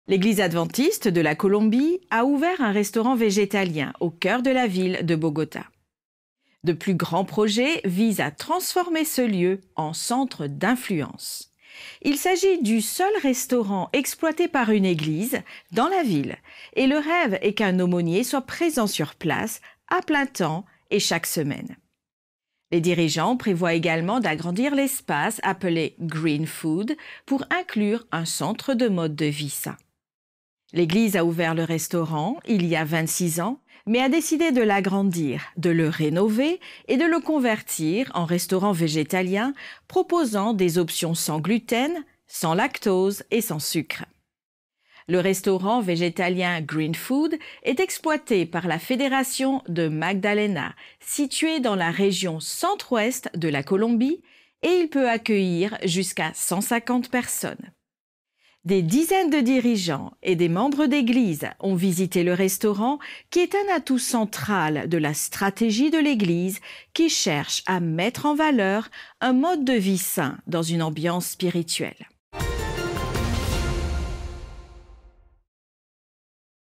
Un restaurant végétalien voit le jour au cœur de la ville de Bogota, en Colombie
Les adventistes de Colombie inaugurent le restaurant végétalien Green Food, récemment rénové.

Les responsables régionaux espèrent transformer le restaurant en un centre d'influence où les membres de la communauté pourront découvrir les valeurs d'un mode de vie sain et spirituel.

Il s'agit du seul restaurant géré par l'Église à Bogota.

ANN est la chaîne d'information officielle de l'Église adventiste du septième jour.